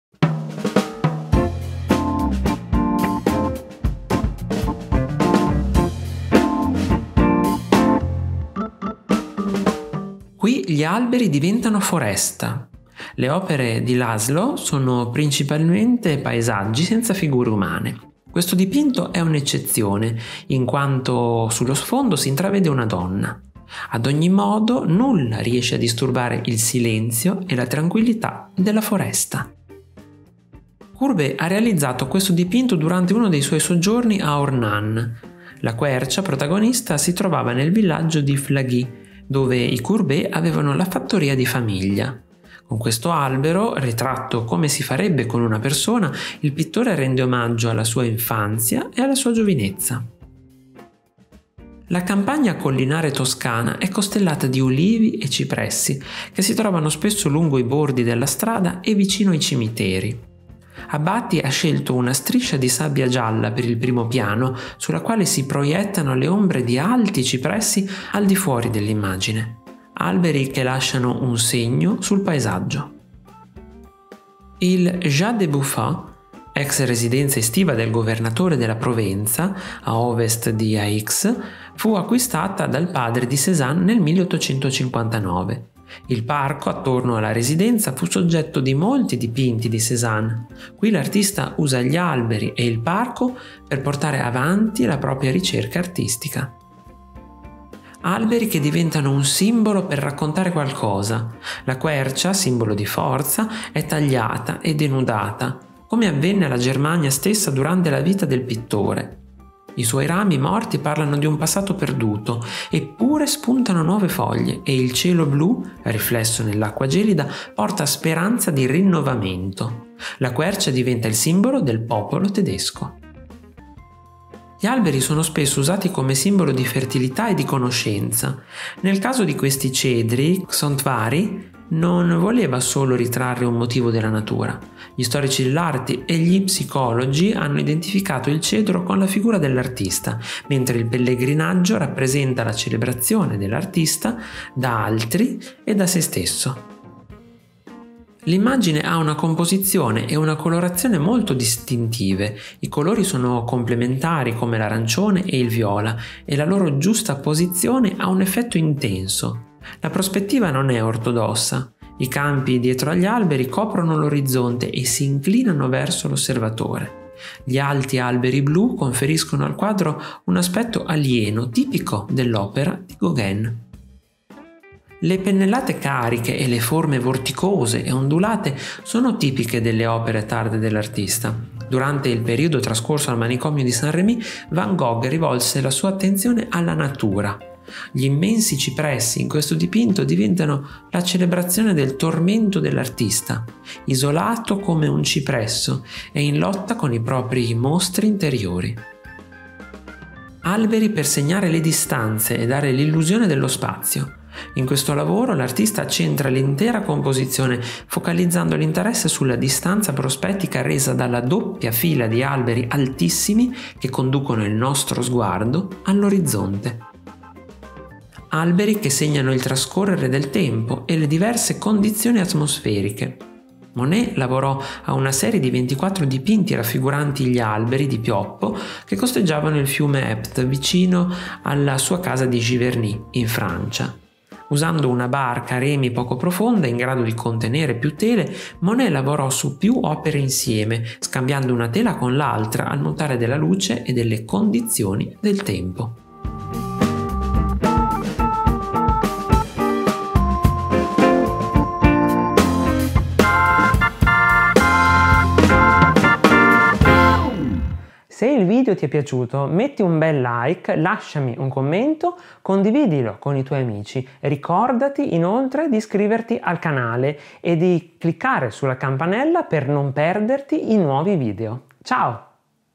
10 momenti di alberi
10 momenti di alberi, alla scoperta di grandi e piccoli capolavori della storia dell'arte a guidarci in un percorso originale ▼▼CONTINUA SOTTO▼▼

Gli alberi sono un elemento fondamentale del paesaggio e, anche per questo, gli artisti li hanno rappresentati molte volte nel corso della storia dell'arte. Alberi che diventano un simbolo. Alberi che ci raccontano una storia. Ma anche alberi ritratti come delle persone. Qui ho realizzato per voi una piccola selezione di 10 alberi per condurvi in un viaggio inconsueto nella storia dell'arte. Un viaggio che spero posa stuzzicare la vostra curiosità e spingervi ad approfondire l'argomento.

Troverai un breve commento alle seguenti opere:
➡ Laszlo Paal, la profondità della foresta, 1877
➡ Gustave Courbet, la quercia di Flagey, 1864
➡ Giuseppe Abbati, strada campestre con cipressi, 1860
➡ Paul Cezanne, alberi nel Jan de Bouffan, 1875-76
➡ Caspar David Friedrich, quercia nella neve, 1829
➡ Tivadar Csontvary Kosztka, pellegrinaggio ai cedri in Libano, 1907
➡ Paul Gauguin, alberi blu, 1888
➡ Vincent van Gogh, cipressi, 1889
➡ Meyndert Hobbema, strada a Middelharnis, 1689
➡ Claude Monet, tre pioppi estate, 1891

LETTURE CONSIGLIATE
➡ La vita segreta degli alberi
➡ Il giro del mondo in 80 alberi